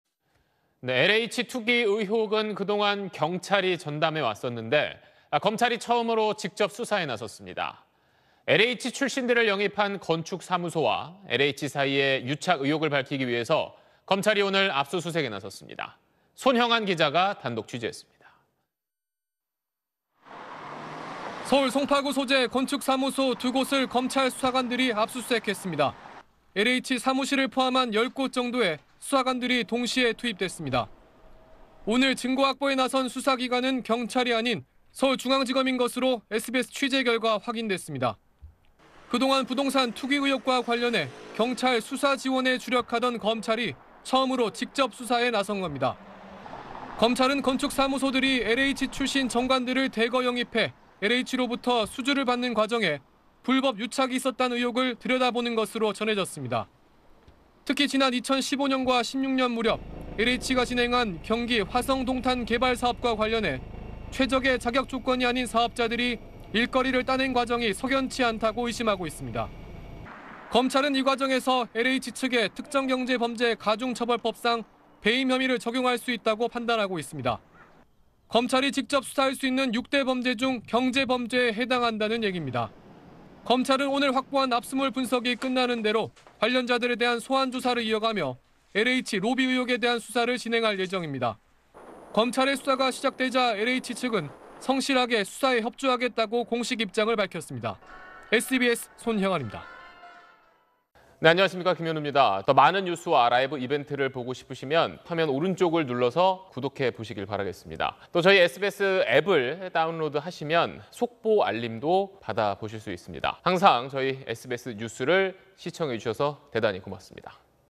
[단독] 검찰도 직접수사 착수…LH · 건축사무소 압수수색 / SBS
LH 투기 의혹은 그동안 경찰이 전담해왔었는데, 검찰이 처음으로 직접수사에 나섰습니다. LH 출신들을 영입한 건축사무소와 LH 사이의 유착 의혹을 밝히기 위해서 검찰이 오늘(11일) 압수수색에 나섰습니다.
손형안 기자가 단독 취재했습니다.